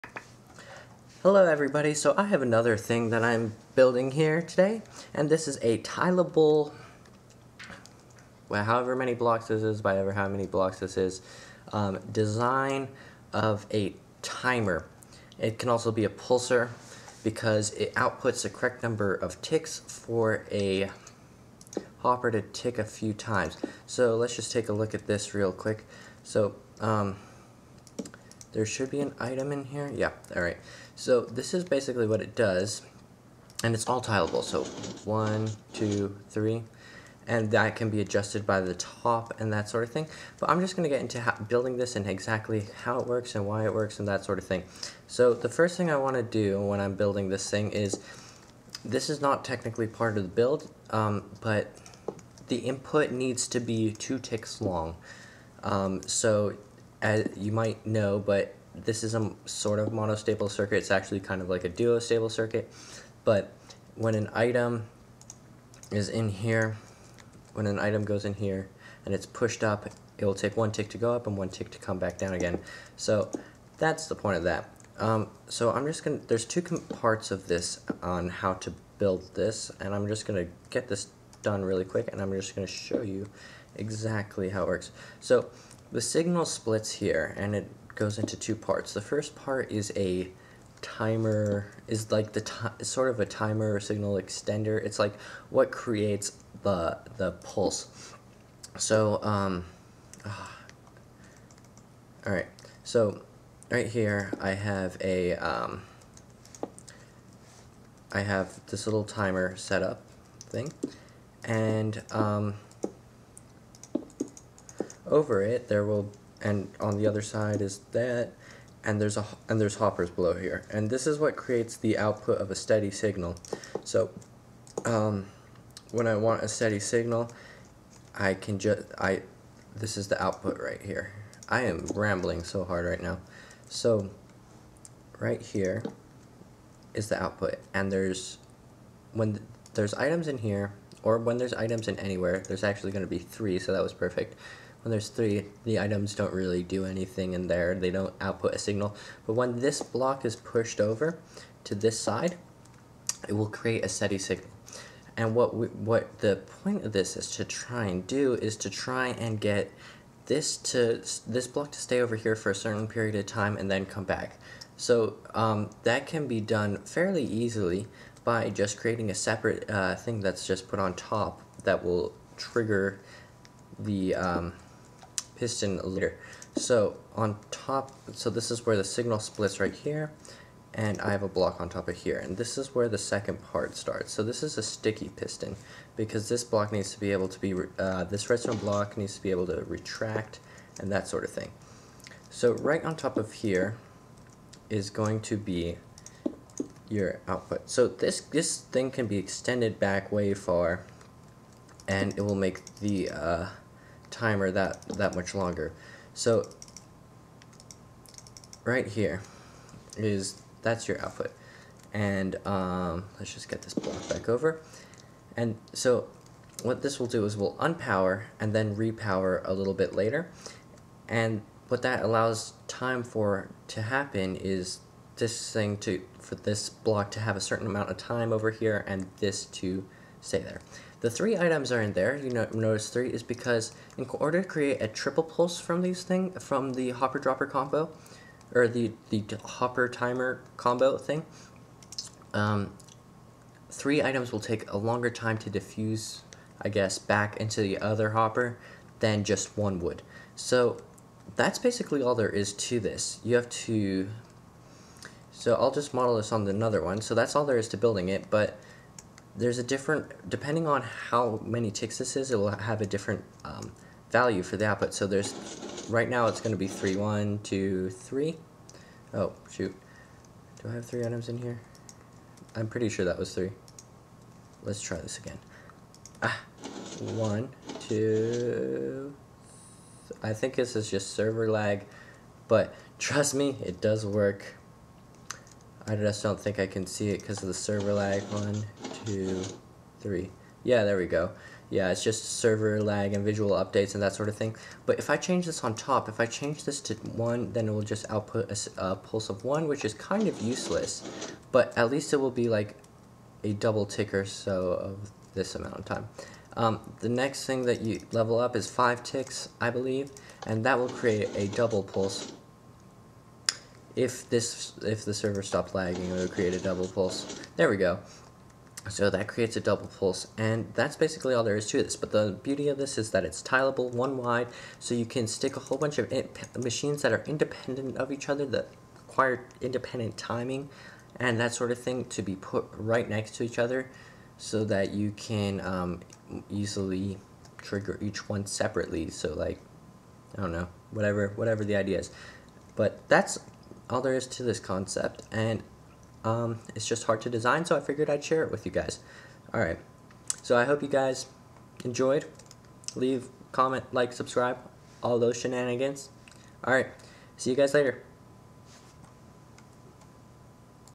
1 wide tileable triple pulser
I show today how to make a one wide tileable pulser, very useful for things with their own indpendent circuts